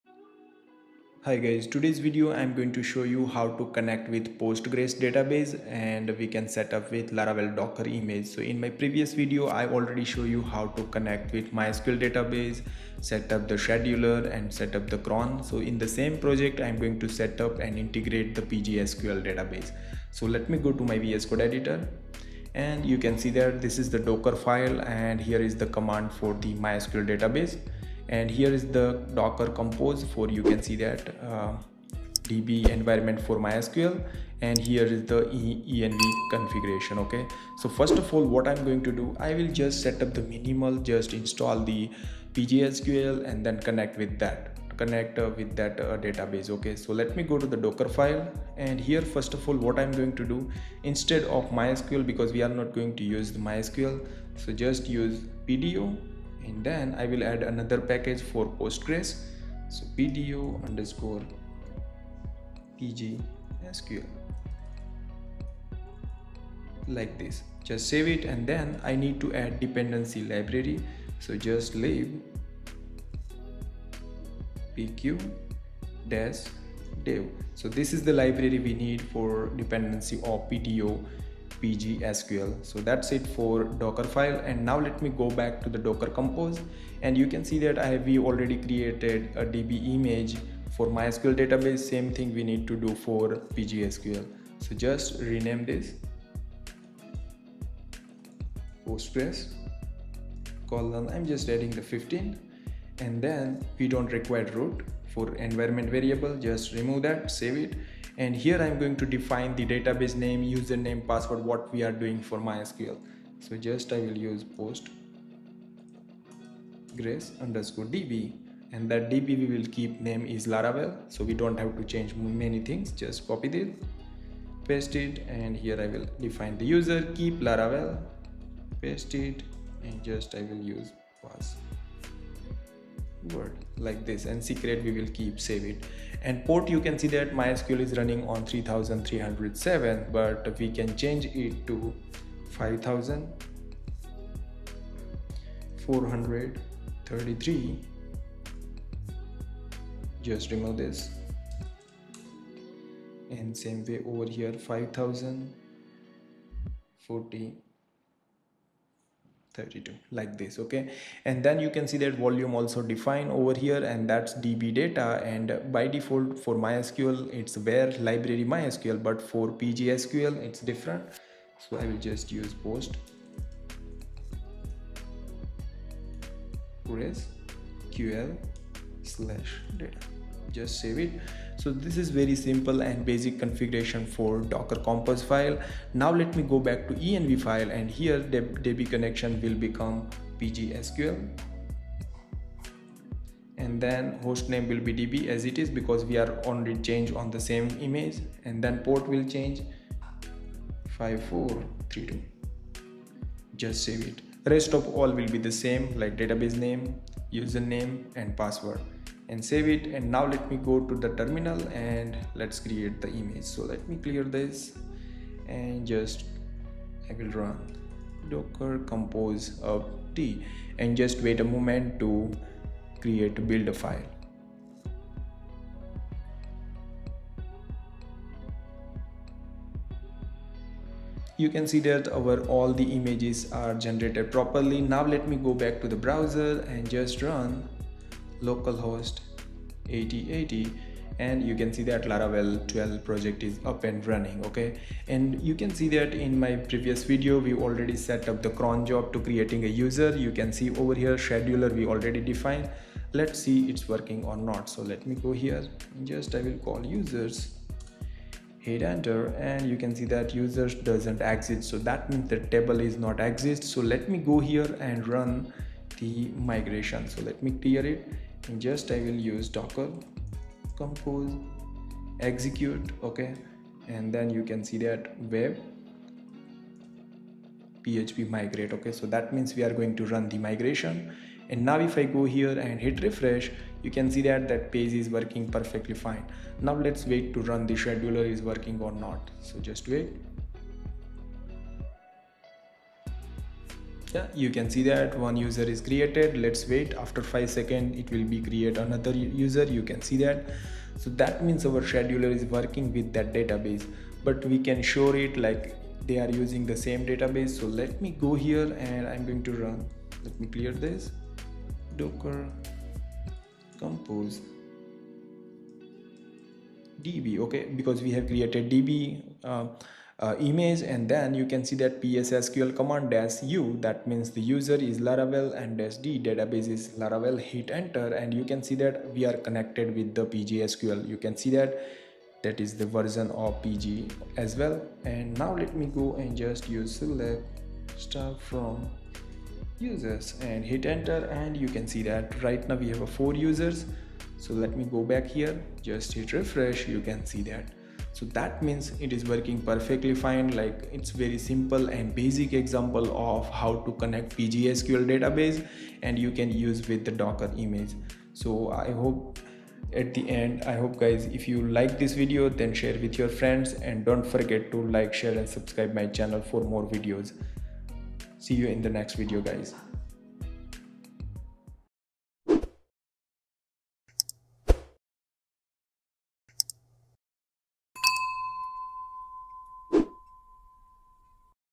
Laravel Docker #6 - Connect Laravel with PostgreSQL Database
Learn how to connect Laravel with PostgreSQL in Docker step by step. 🚀 In this tutorial, we’ll configure the database, update environment settings, and make sure your Laravel app runs smoothly with PostgreSQL inside Docker.
🔹 What you’ll learn:
* Configure PostgreSQL in Docker
* Connect Laravel to PostgreSQL database
* Best practices for database setup in Docker
👉 Perfect for developers working with Laravel + Docker + PostgreSQL.